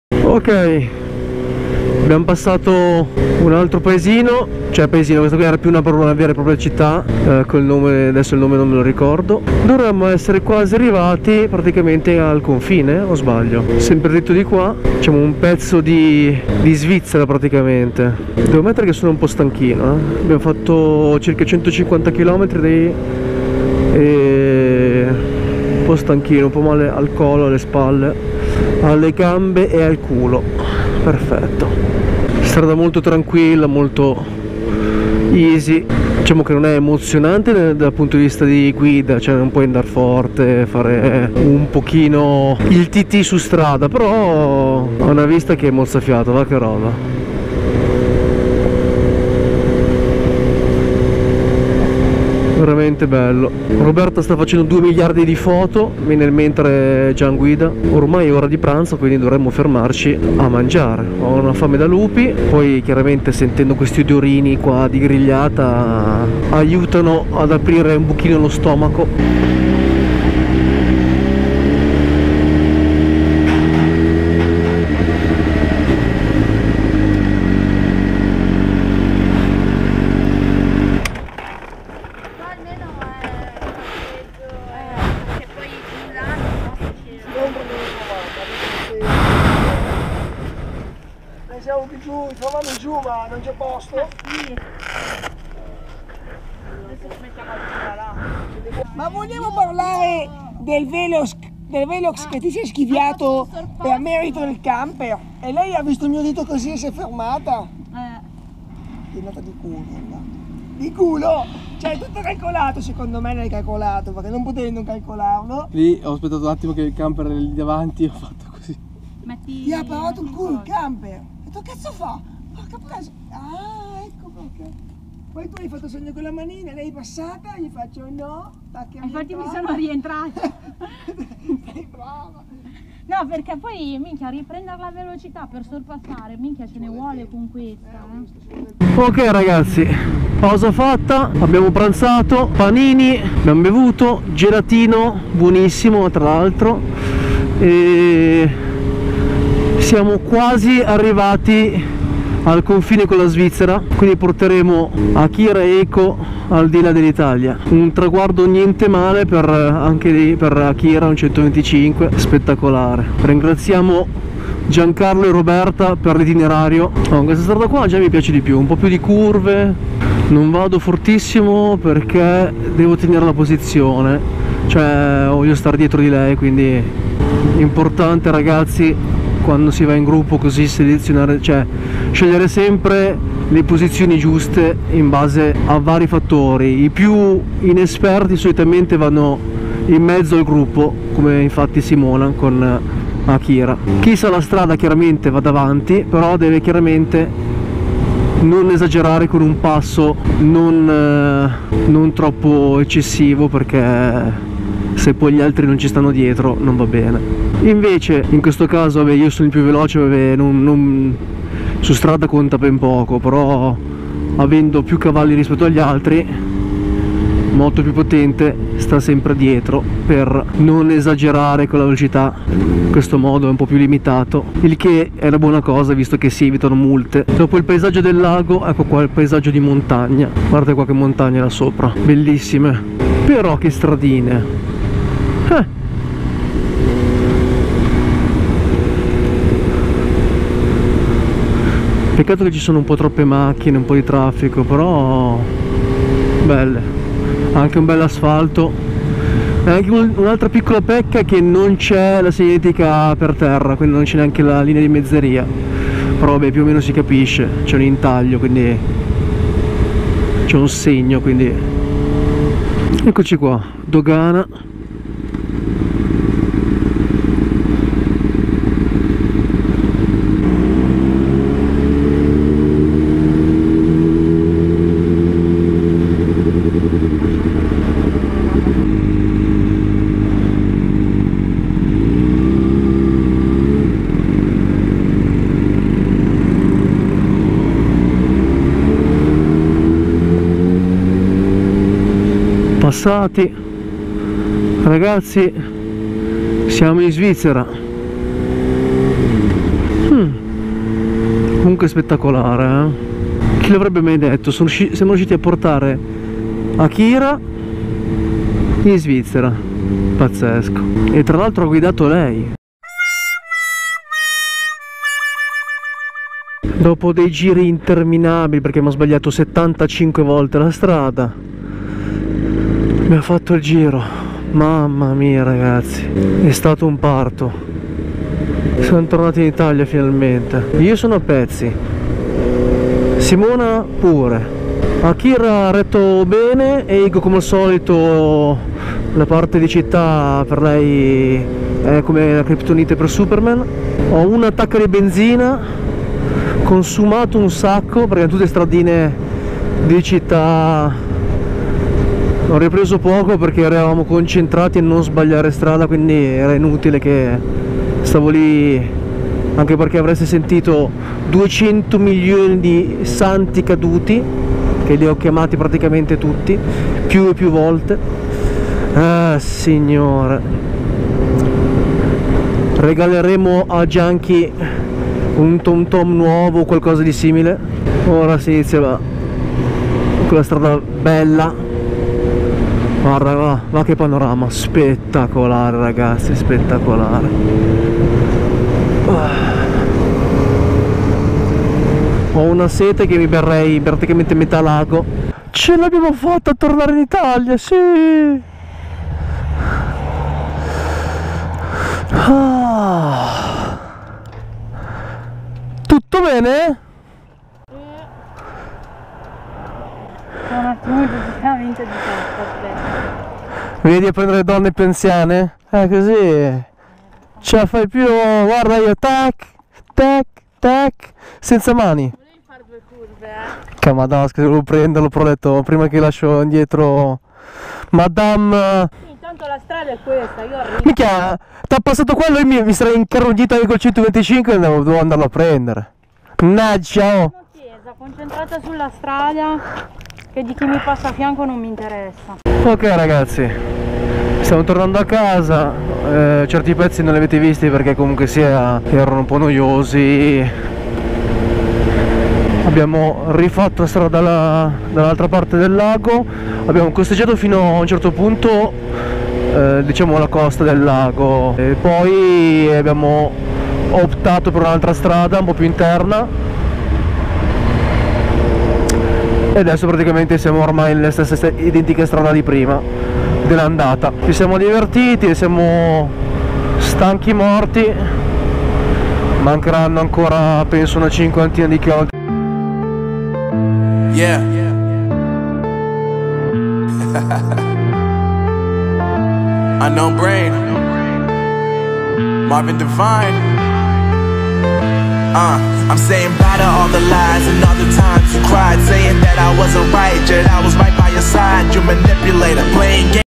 Motovlog #21 parte 2: abbiamo sconfinato !!!
Eccola qui la seconda parte del giro di ferrafgosto.
Qualche piccolo intoppo ma alla fine è stato un gran bel viaggio.
Buona visione